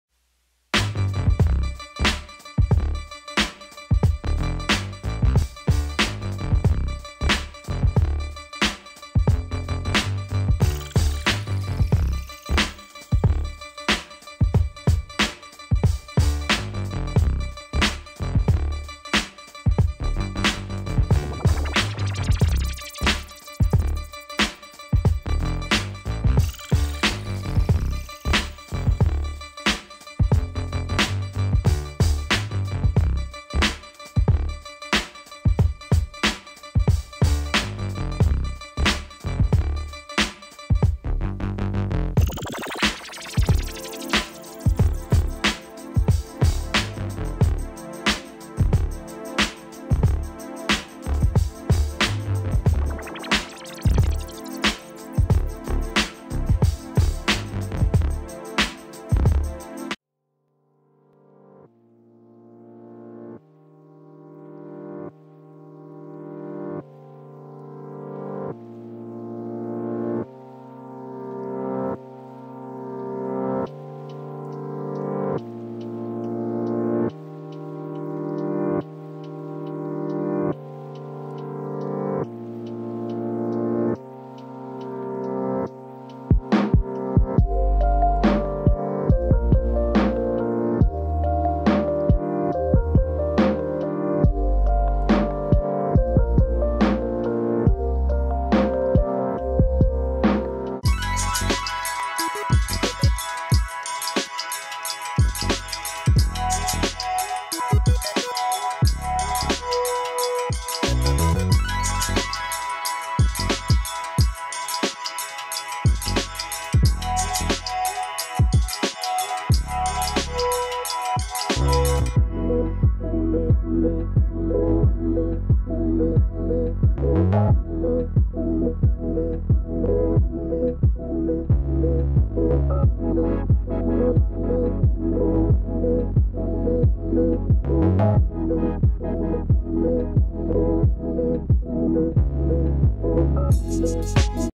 SPRAY PAINT ART! - Mario Galaxy and Into The Spider-Verse
Watch me paint Rosalina, Mario, and Miles Morales in this spray paint art compilation!